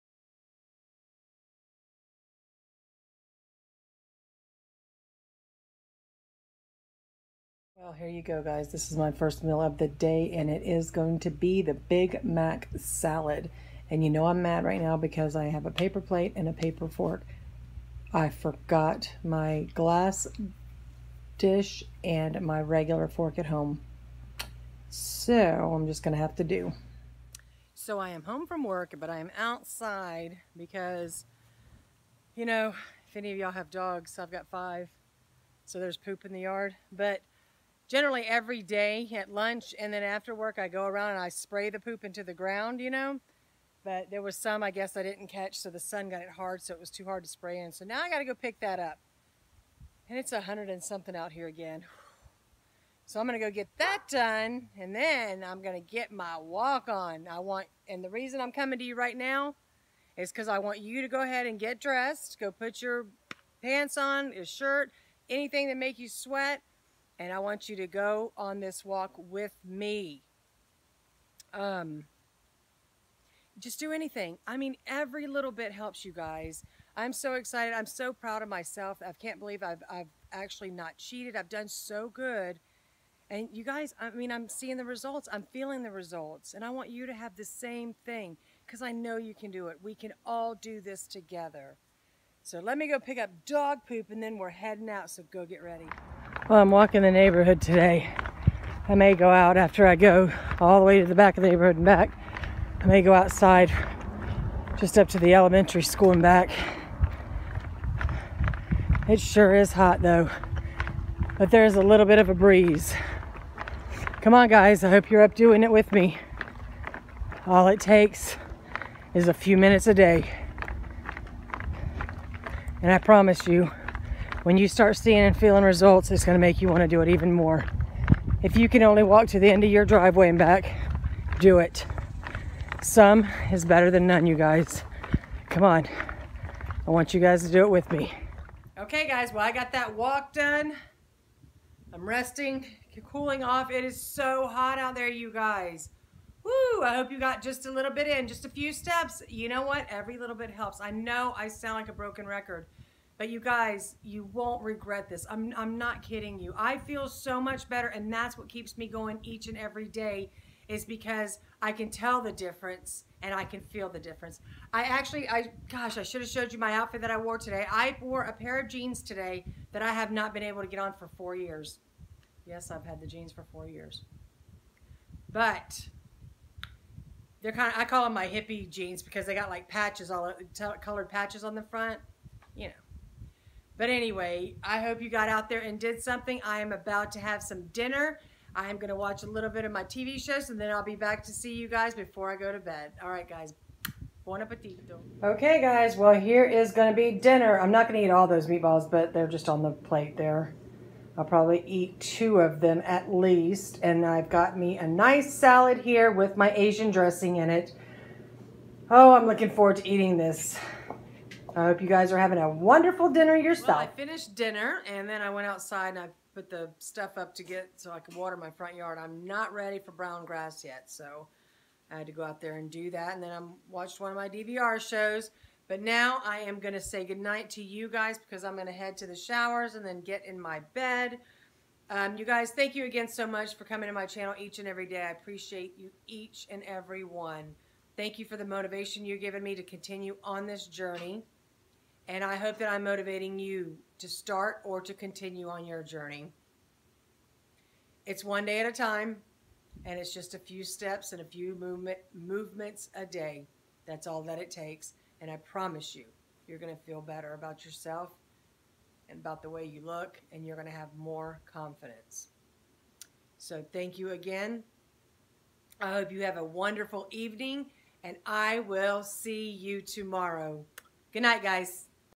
Day 21-Less than a week to go Keto Challenge!
Lose 20 in 28 Days Keto Challenge! Sounds impossible and maybe it is but I feel Amazing so far!! 💖💖